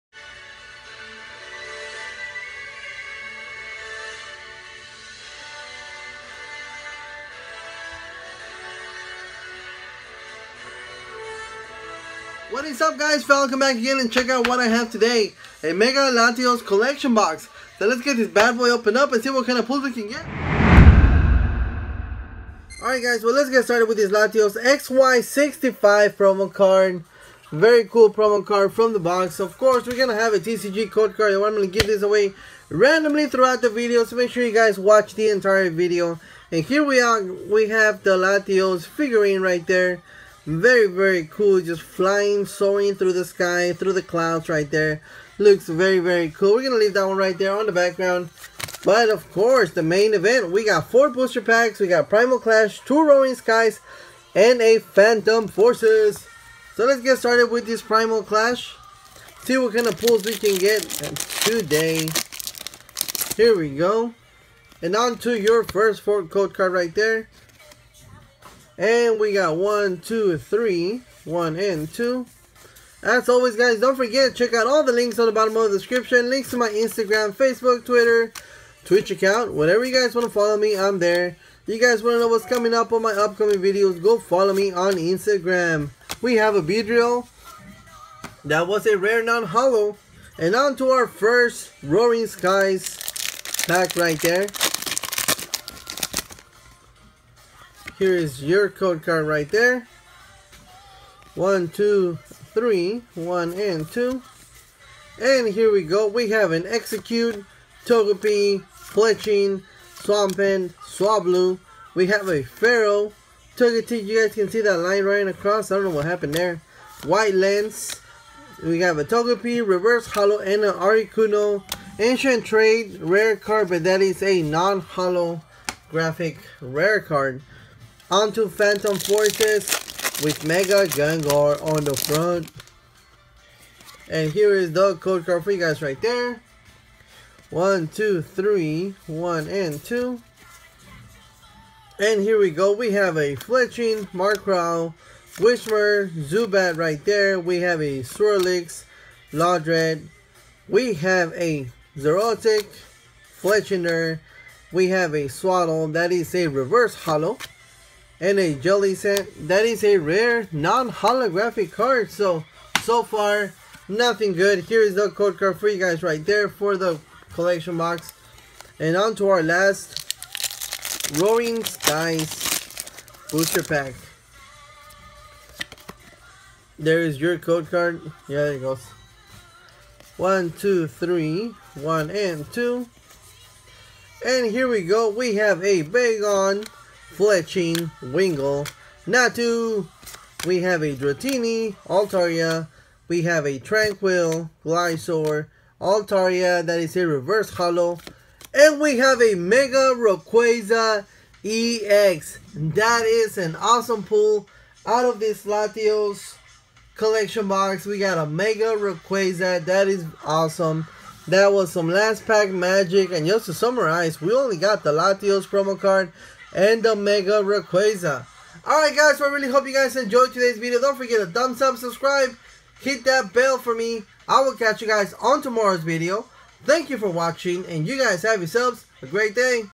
AWESOME MEGA PULL!! Opening A Pokemon Mega Latios Collection Box!!!!
A Pokemon Mega Latios Collection box is opened in this video. This box includes a Mega Latios figurine, Latios Black Star Promo holofoil card, online TCG code card, and 4 Pokemon booster packs, 2 Roaring Skies, and 1 each of Primal Clash and Phantom Forces. I bought this Mega Latios Collection box from Walmart for $19.99.

Intagram.